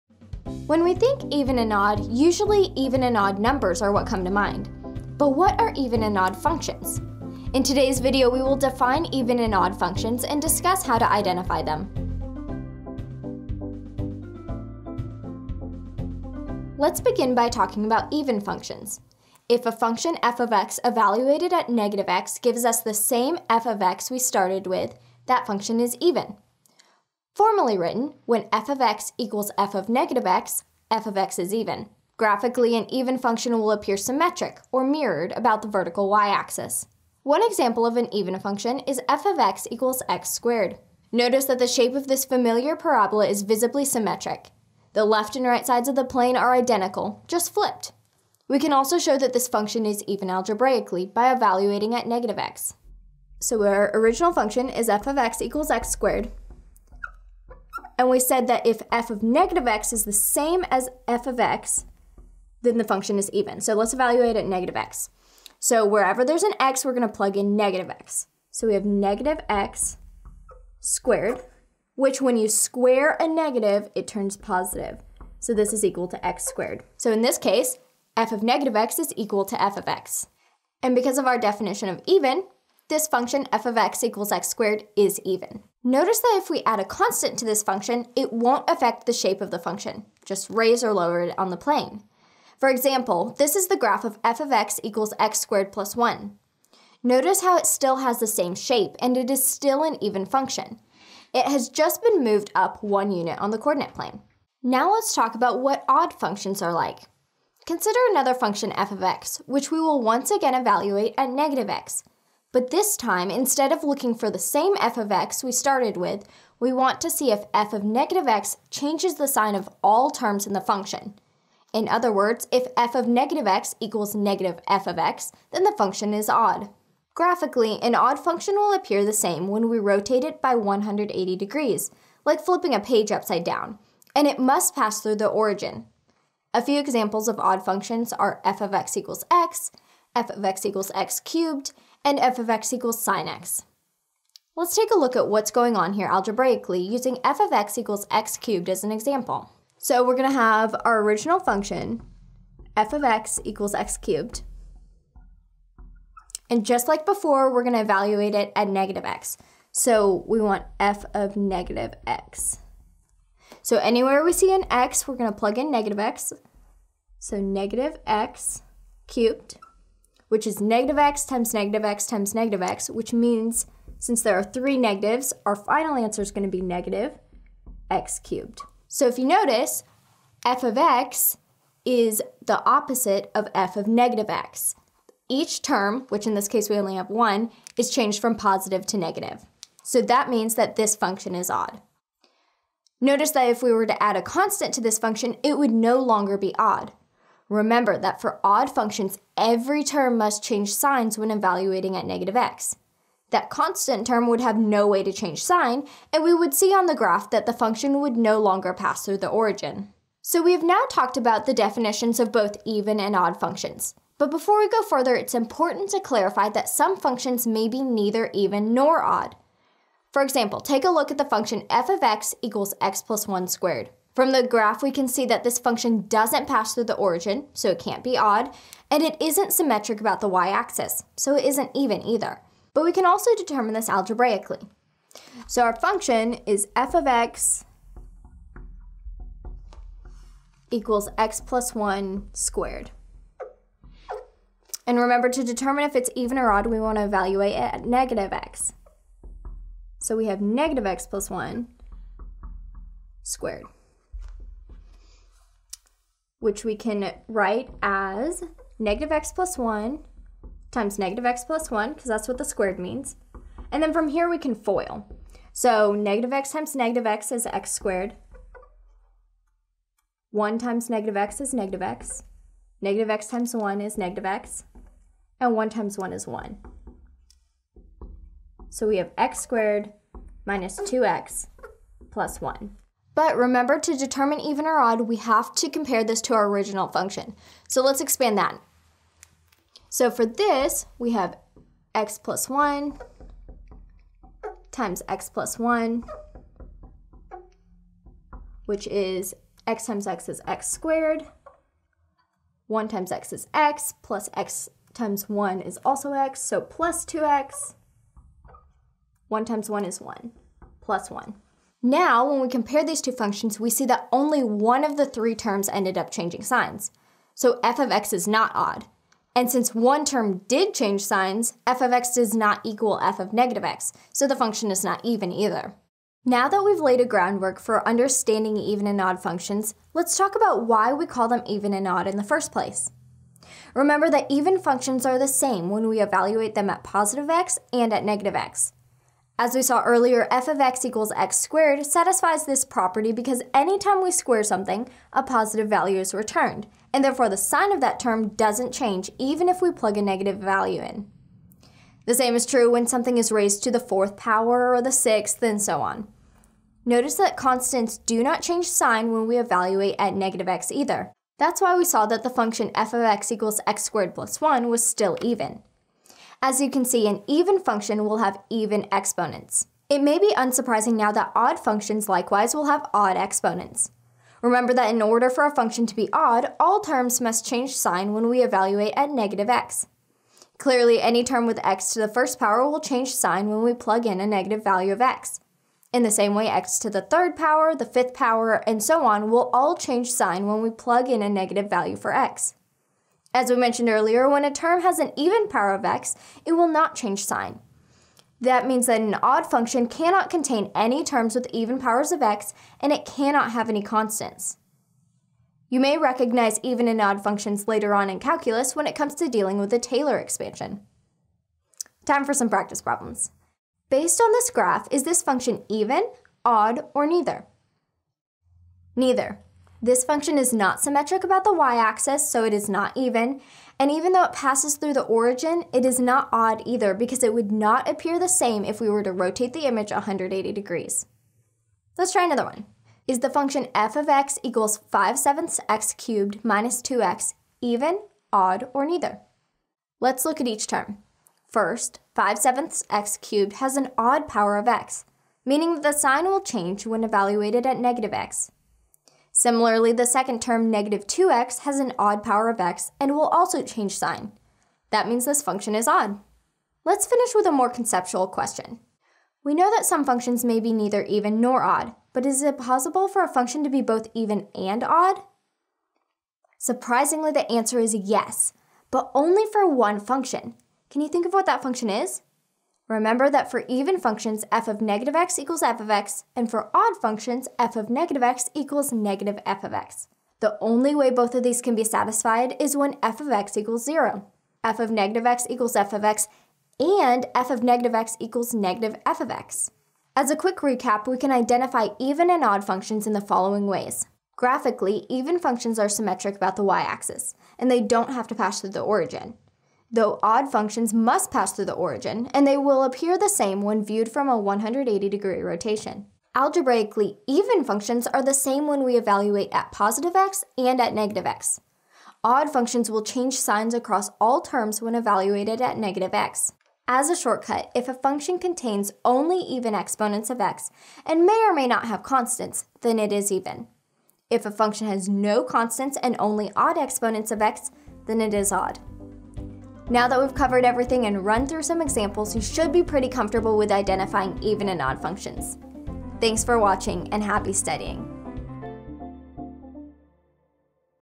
Even and Odd Functions
When we think “even and odd,” usually even and odd numbers are what come to mind. But what are even and odd functions? In today’s video, we will define even and odd functions and discuss how to identify them.